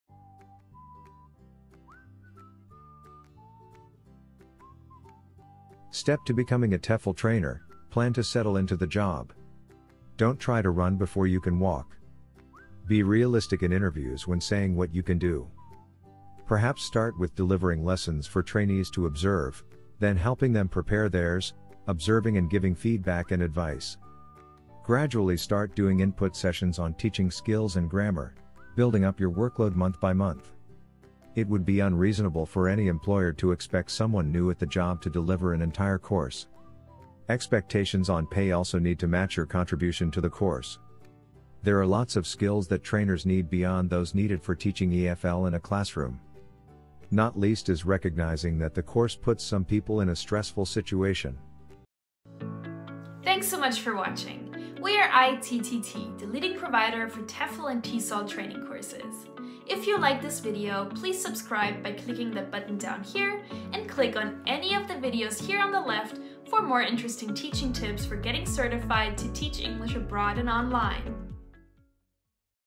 How to become TEFL Teacher? - Plan to settle into the job
Do you want to become a TEFL teacher? Then, this video is for you. You'll learn what is important to know before starting a TEFL course and building a successful TEFL teacher career.